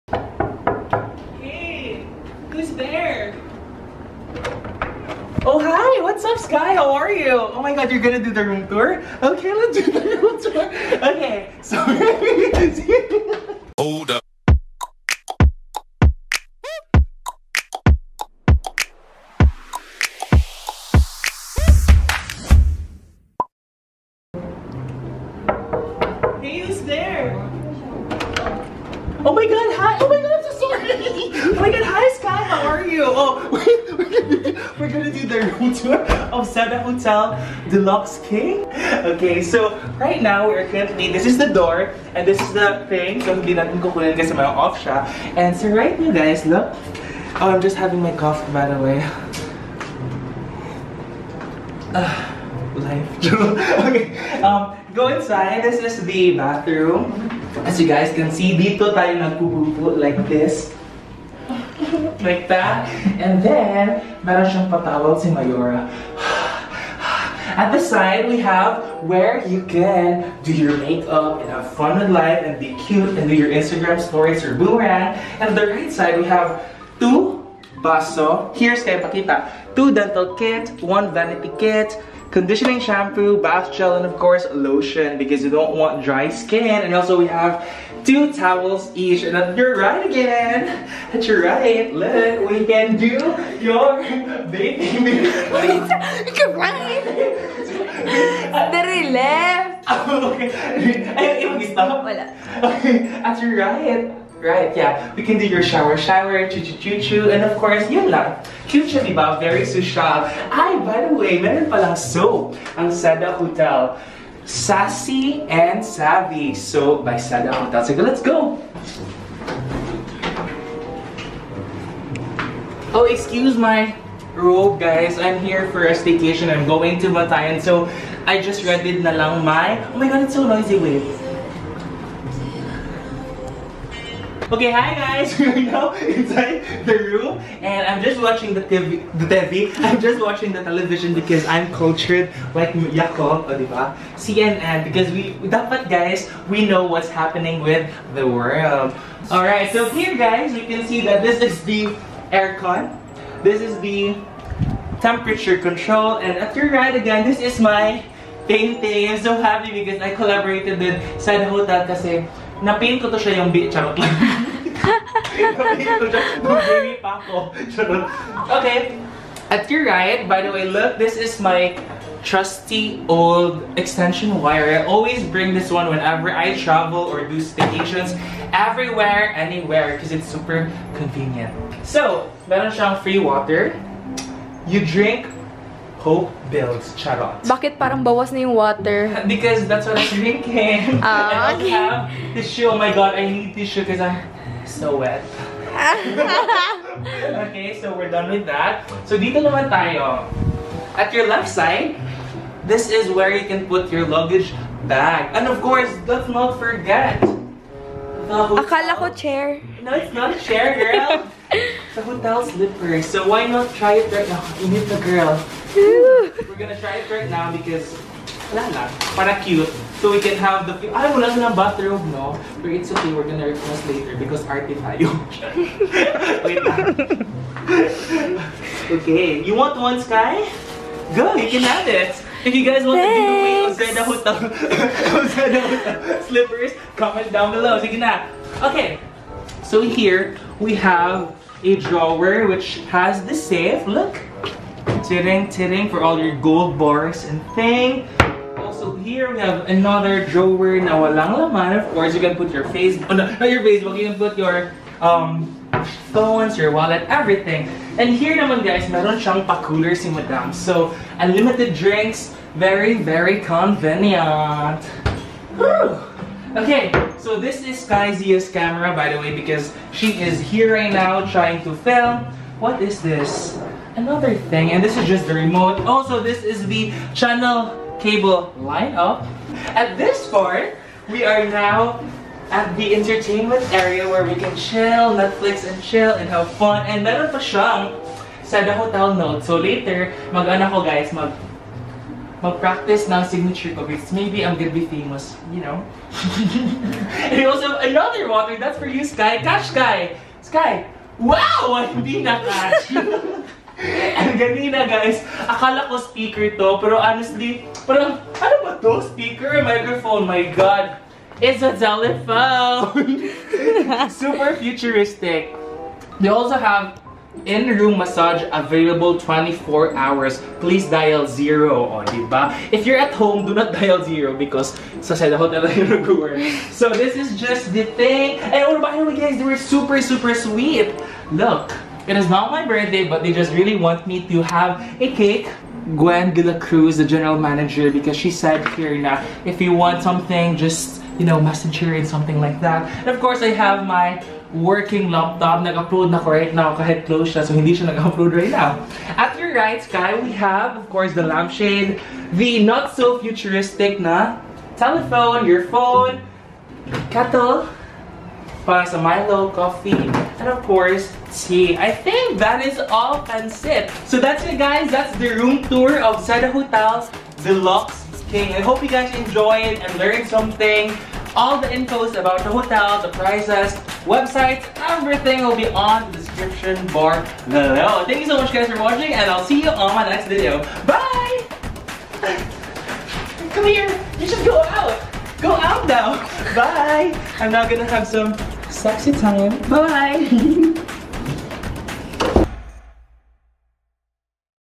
ROOM TOUR 2019! | Christian Layese X Seda Hotel Cebu City (Philippines)
Today I'll be showing you the room in Seda Hotel Cebu, The deluxe King. Price is 6,800 Philippine Pesos / 129 USD. The Deluxe King ia s Modern, flexible space equipped with requisite amenities for today’s traveler.

- Discover a new level of comfort, service and convenience that’s uniquely Seda right here in Cebu City, “Queen City of the South.” Amid a thriving economy and prosperous business landscape, discover monuments to a colorful past and festive cultural traditions.

The 301-room hotel merges past and present through a seamless transition from its beginnings as a key player in the development of the city’s financial district to its re-birth as Seda Ayala Center Cebu.